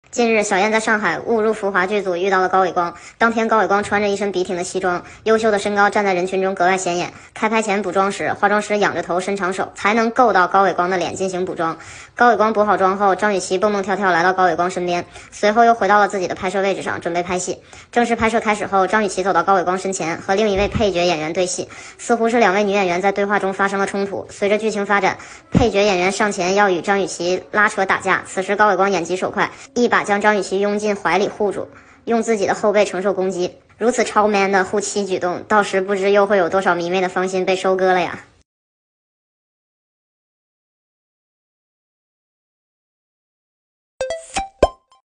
20210301 电视剧误入浮华# 张雨绮、高伟光 片场路透，苏沫与人发生冲突，王居安眼疾手快，一把将苏沫拥进怀里护住，实力“护妻”超MAN！
电视剧误入浮华# 张雨绮、高伟光 片场路透，苏沫与人发生冲突，王居安眼疾手快，一把将苏沫拥进怀里护住，实力“护妻”超MAN！[good]